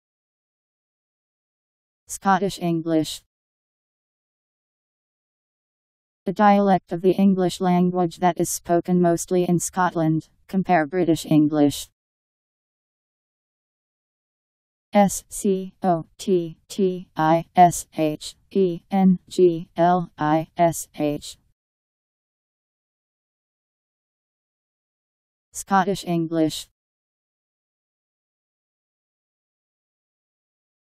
Scottish English Meaning
Video shows what Scottish English means. A dialect of the English language that is spoken mostly in Scotland. Compare British English..  Scottish English Meaning. How to pronounce, definition audio dictionary. How to say Scottish English. Powered by MaryTTS, Wiktionary